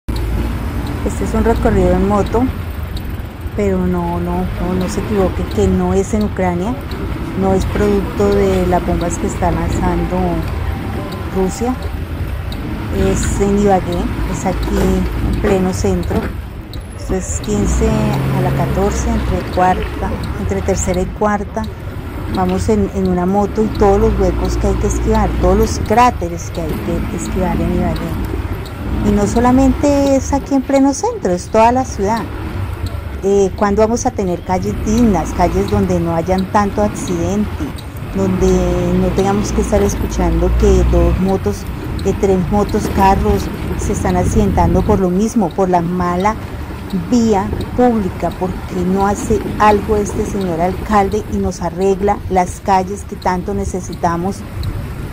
“¿Cuándo vamos a tener calles dignas que no generen accidentes?”: es lo que reclama una motociclista
Siguen las quejas de algunos ciudadanos respecto al mal estado en que se encuentran gran parte de las vías en Ibagué, bien sea por accidentes, problemas de movilidad, afectación a negocios comerciales, entre otras situaciones, cada vez son más constantes las denuncias y quejas airadas contra la administración municipal y su aparente falta de respuesta frente a estas problemáticas.